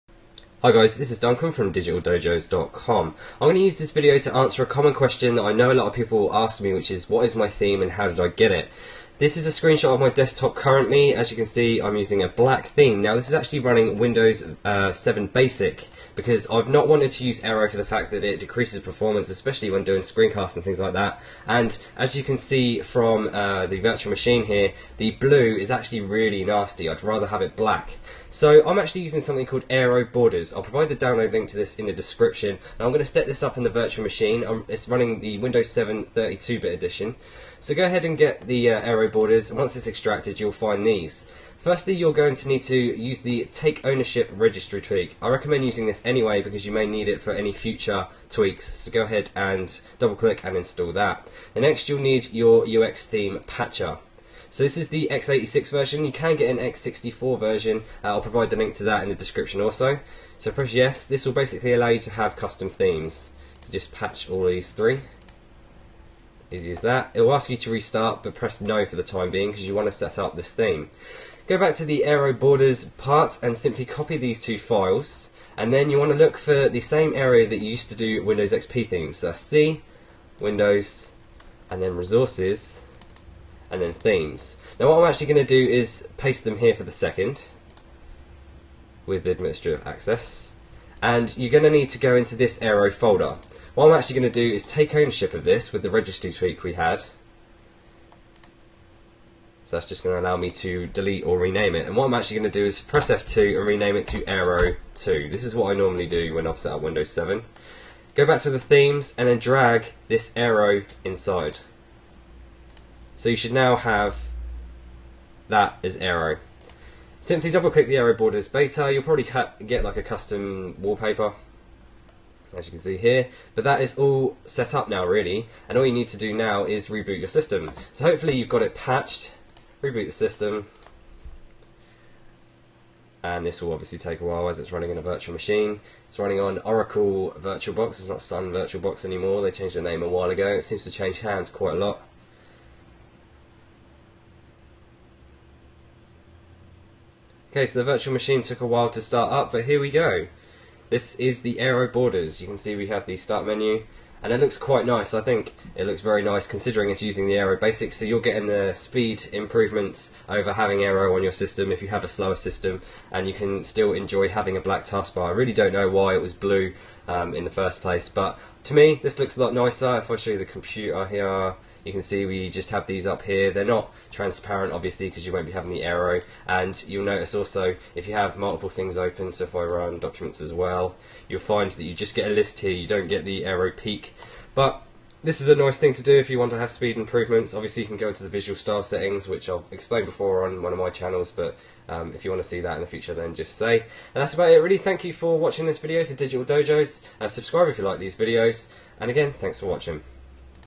Duncan's Windows 7 Basic Aero Style Black Theme
This video is dedicated to those asking for my theme and how I have it. If someone asks this, please point them here :-)

This Video was powered by Camtasia Studio kindly provided by Techsmith Connect with DigitalDojos